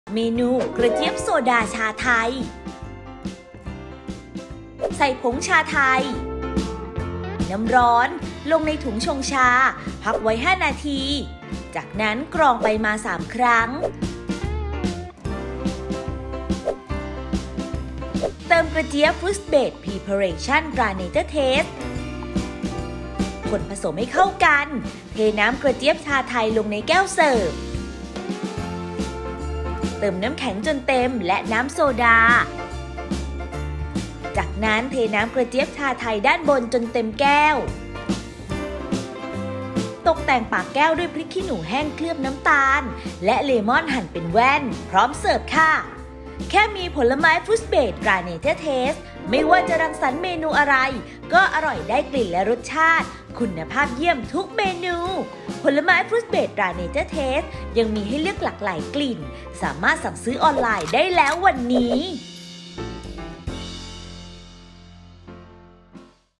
กระเจี๊ยบโซดาชาไทย สูตรเด็ด ทำง่าย สไตล์ "อ.ยิ่งศักดิ์"
กระเจี๊ยบโซดาชาไทย

ผลิตภัณฑ์ : กระเจี๊ยบ ฟรุ๊ตเบส พรีเพอเรชั่น ตราเนเจอร์เทส

ผงชาไทย 20 กรัม
กระเจี๊ยบ ฟรุ๊ตเบส พรีเพอเรชั่น ตราเนเจอร์เทส 150 มิลลิลิตร
น้ำแข็งก้อนเล็ก 150 กรัม
น้ำโซดา 60 มิลลิลิตร
แก้วสำหรับเสิร์ฟ
พริกขี้หนูแห้งเคลือบน้ำตาล ,เลม่อนหั่นเป็นแว่น สำหรับแต่ง

1.ใส่ผงชาไทย น้ำร้อนลงในถุงชงชา พักไว้ 5 นาที จากนั้นกรองไปมา 3 ครั้ง
เติมกระเจี๊ยบ ฟรุ๊ตเบส พรีเพอเรชั่น ตราเนเจอร์เทส คนผสมให้เข้ากัน
2.เทน้ำกระเจี๊ยบชาไทยลงในแก้วเสิร์ฟประมาณ 1/4 ของแก้ว เติมน้ำแข็งจนเต็ม และน้ำโซดาประมาณ 2/3 ของแก้ว จากนั้นเทน้ำกระเจี๊ยบชาไทยด้านบนจนเต็มแก้ว
3.ตกแต่งปากแก้วด้วยพริกขี้หนูแห้งเคลือบน้ำตาล และเลม่อนหั่นเป็นแว่น พร้อมเสิร์ฟ

▲Line @yingsakfood